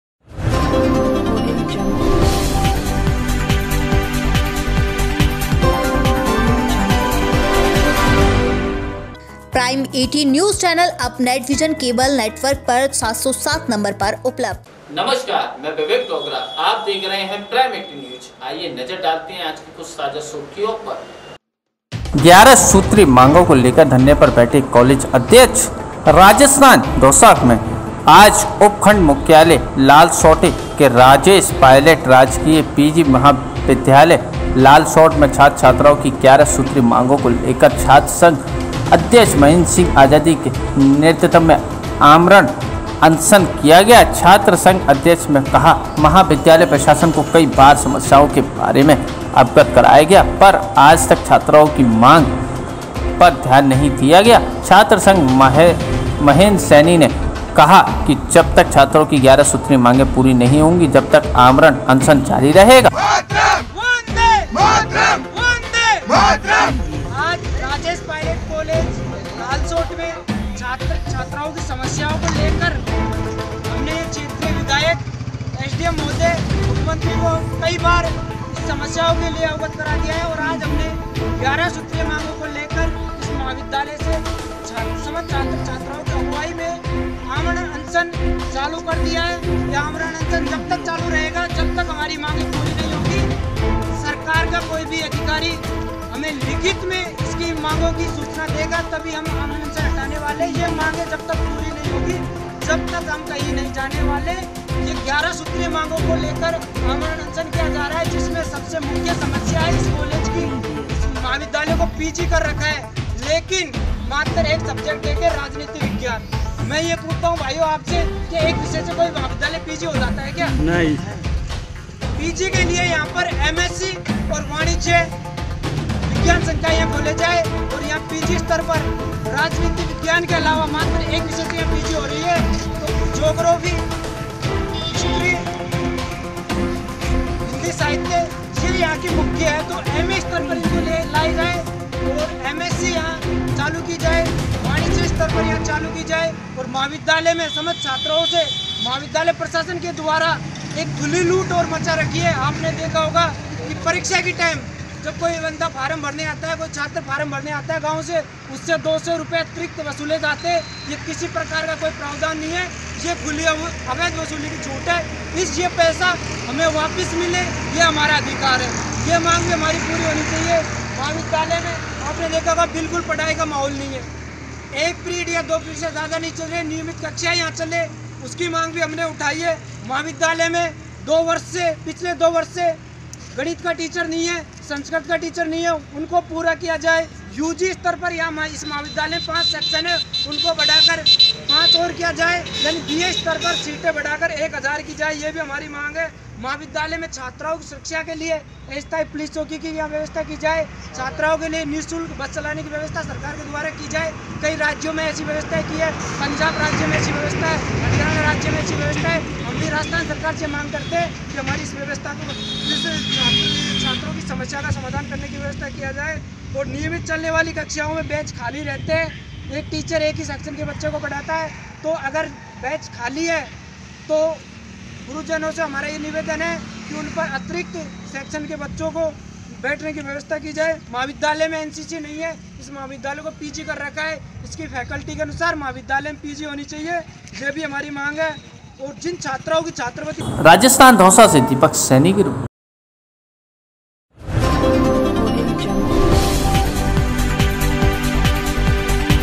11 सूत्री मांगों को लेकर धरने पर बैठे कॉलेज अध्यक्ष
11 सूत्री मांगों को लेकर धरने पर बैठे कॉलेज अध्यक्ष